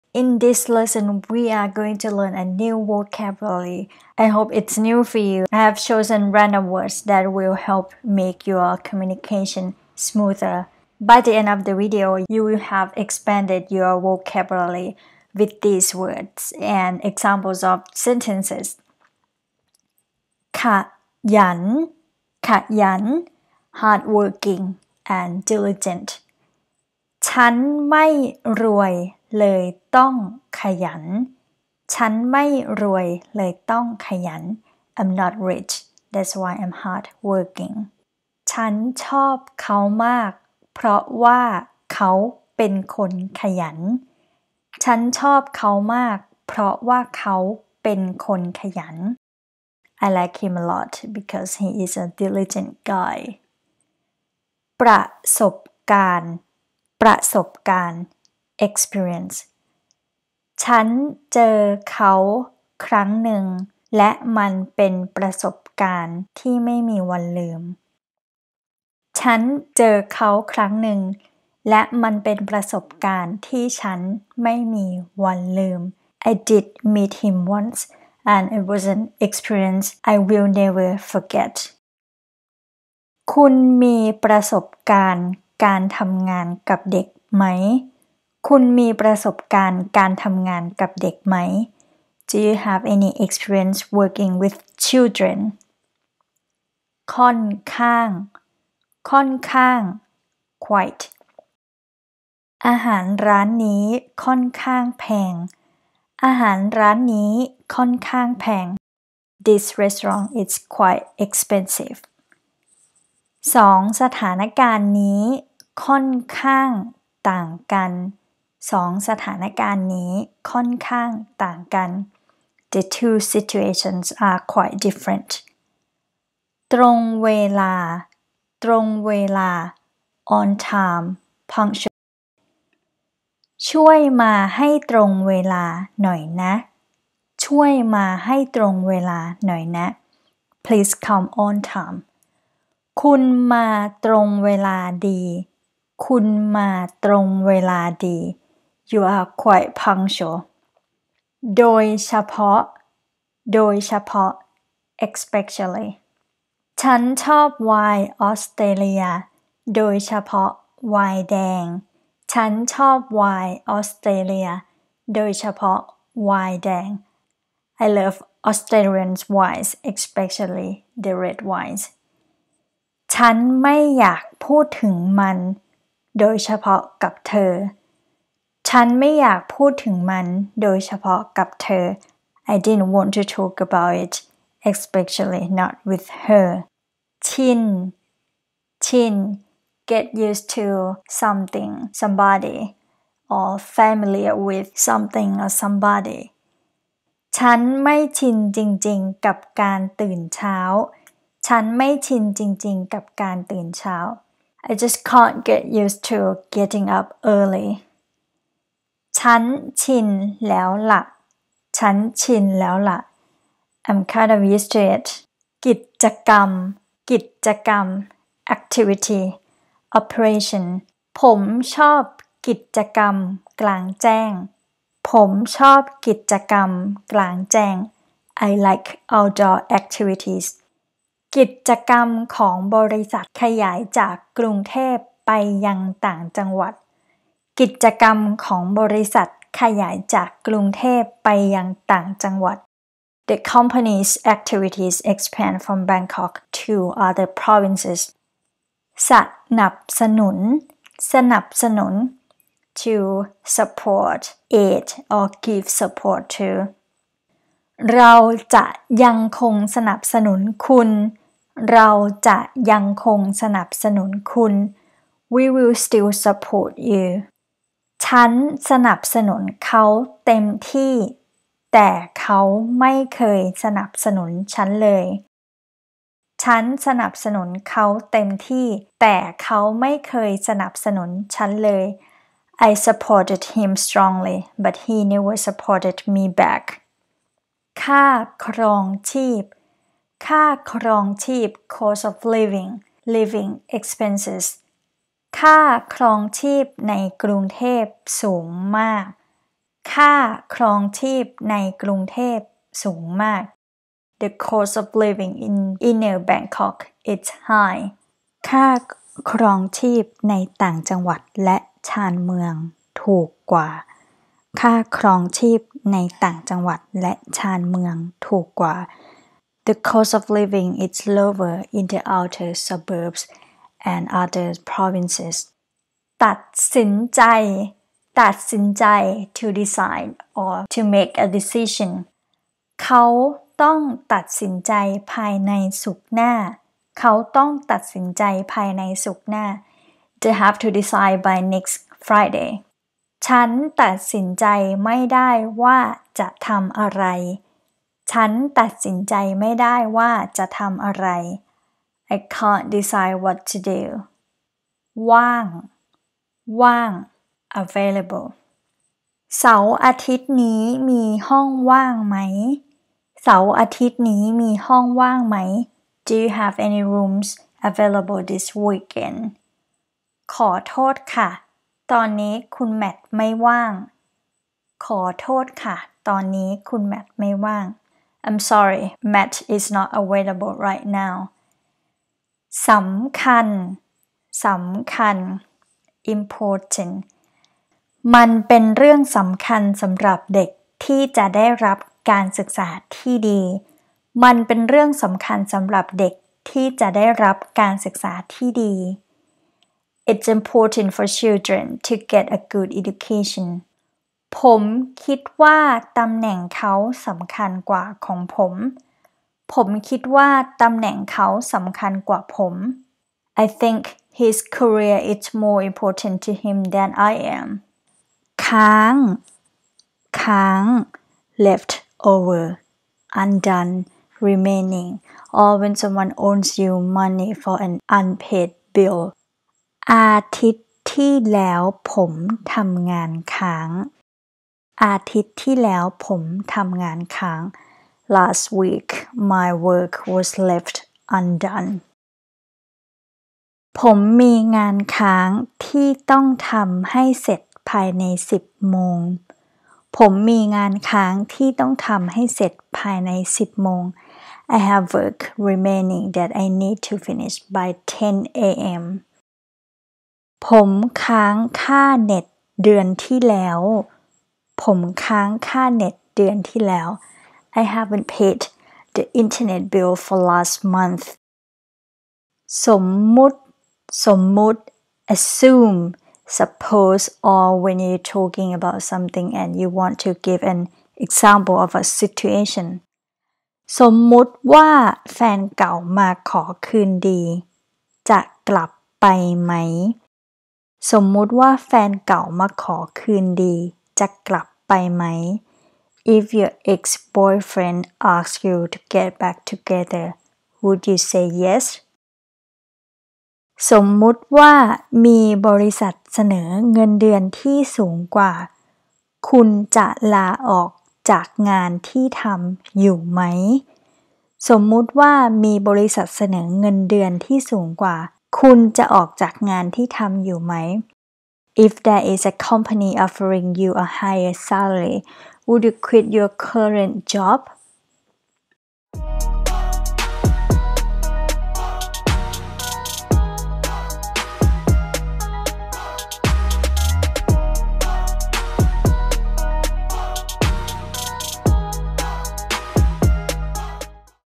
14 Words for INTERMEDIATE Learners | Learn Thai with Shelby
14 Words for INTERMEDIATE Learners

1. ขยัน /khà-yǎn/ = hard-working, diligent
2. ประสบการณ์ /bprà-sɔ̀ːb-gaan/ = experience
3. ค่อนข้าง /kɔ̂ːn-khâang = quite
4. ตรงเวลา /dtrɔːng we-laa/ = on time, punctual
5. โดยเฉพาะ /doi chá-páo/ = especially
6. ชิน /chin/ = get used to sth/sb familiar with
7. กิจกรรม /gìd-jà-gam/ = activity, operation
8. สนับสนุน /sà-nàb-sà-nǔn/ = [to] support, aid, give support to
9. ค่าครองชีพ /kâa krrɔːng chêeb/ = cost of living, living expenses
10. ตัดสินใจ /dtàd sǐn-jai/ = to decide
11. ว่าง /wâang/ =  available
12. สำคัญ  /sǎm-kan/ = important
13. ค้าง /káang/ = left over, undone
14. สมมต /sǒm-múd/ = assume, suppose
𝐂𝐨𝐧𝐭𝐫𝐢𝐛𝐮𝐭𝐞 𝐚𝐧𝐲 𝐚𝐦𝐨𝐮𝐧𝐭 𝐲𝐨𝐮 𝐟𝐞𝐞𝐥 𝐜𝐨𝐦𝐟𝐨𝐫𝐭𝐚𝐛𝐥𝐞 𝐰𝐢𝐭𝐡 👇🏻